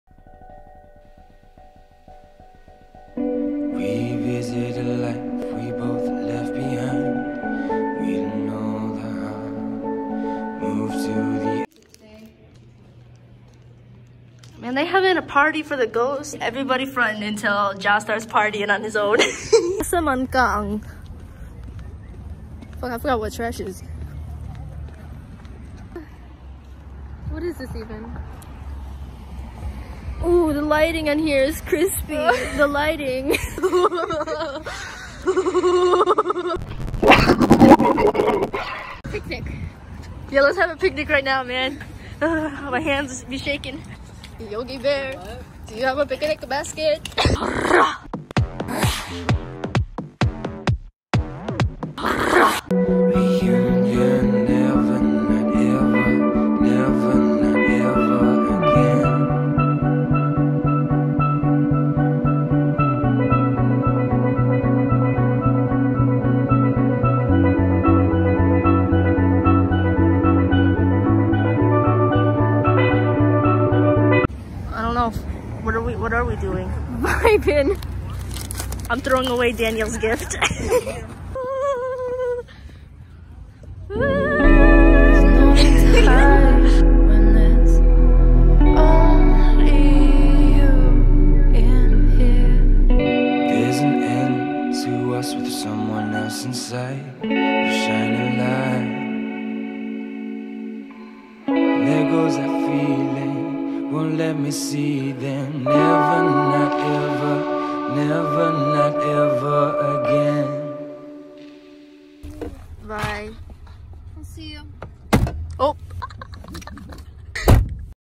Vlog #2
This was December 2019 Downtown Farmington Michigan around Christmas time.